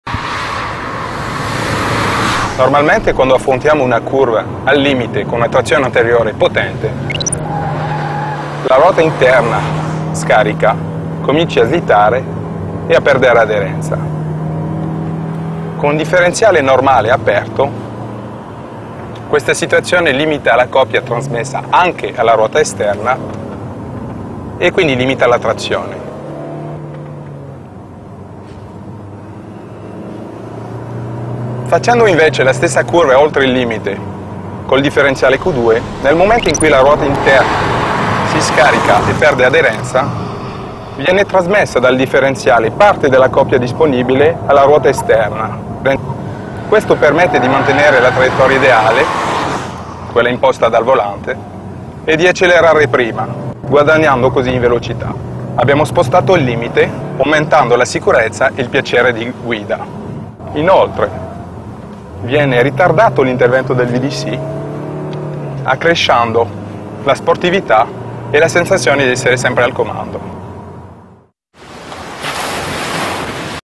Alfa Rmeo Q2
Philippe Krief explains how the Q2 system works